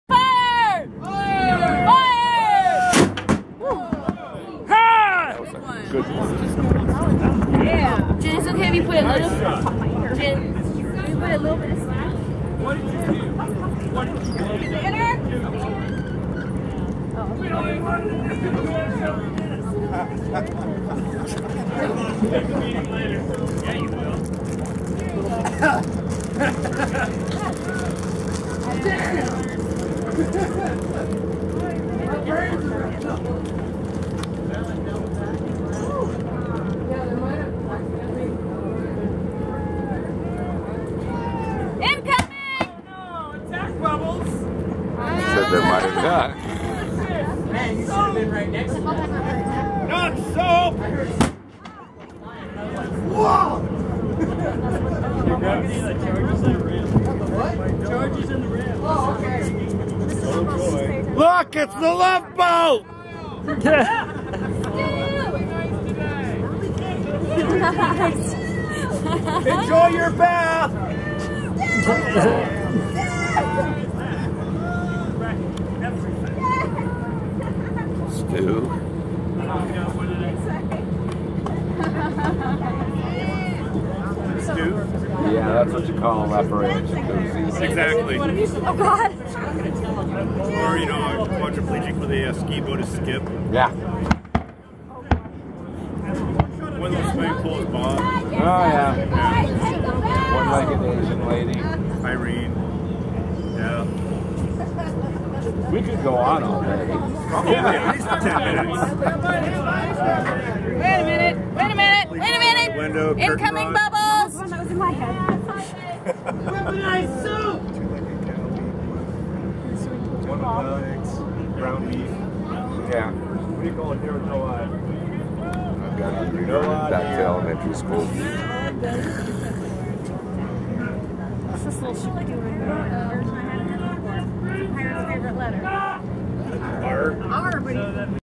Aboard the Hawaiian Chieftain in Port Orchard
We got to go on a 2-hour cruise aboard the Hawaiian Chieftain, and have a mock battle with the Lady Washington at Dead Men Tell No Tales in Port Orchard Washington.